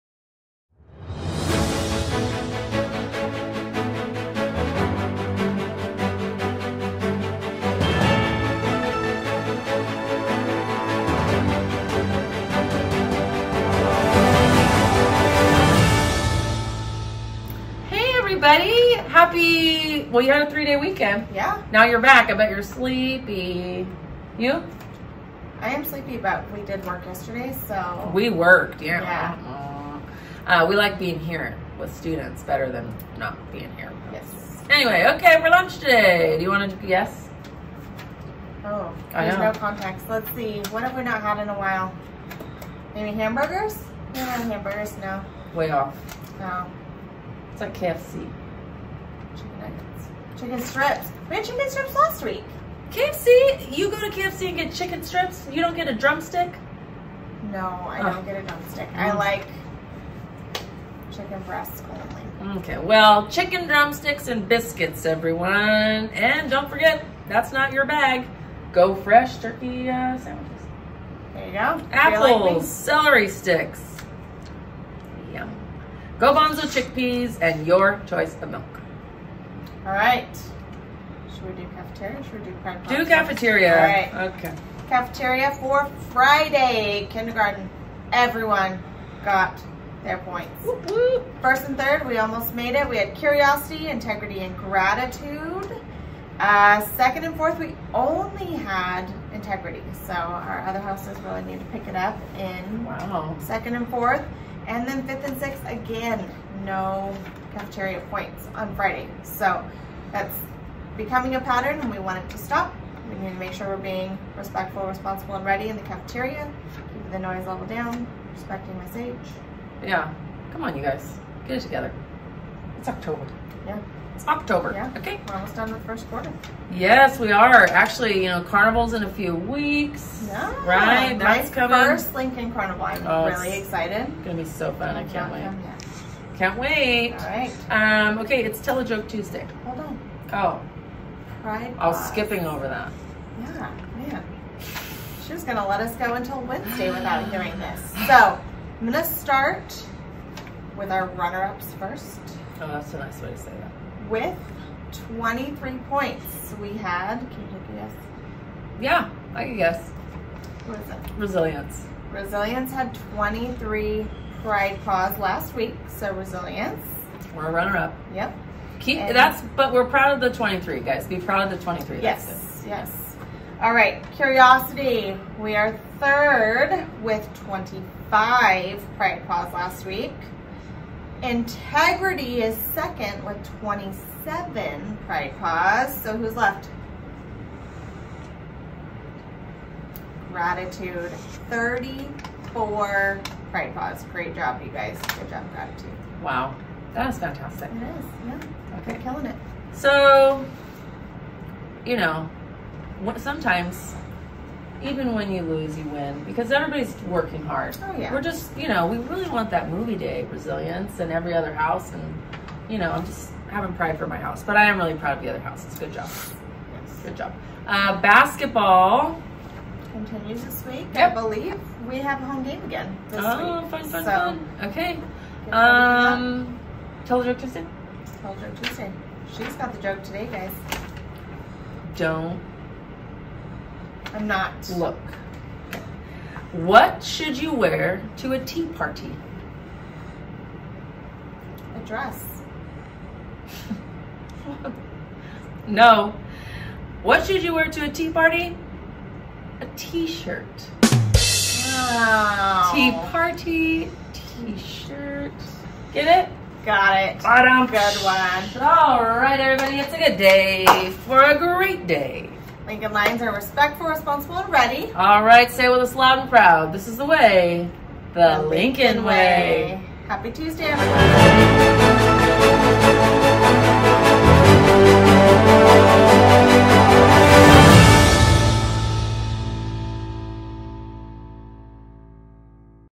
The Lion Roar for Tuesday, October 8, 2024
Please listen to our morning announcements from our Lincoln Administrators.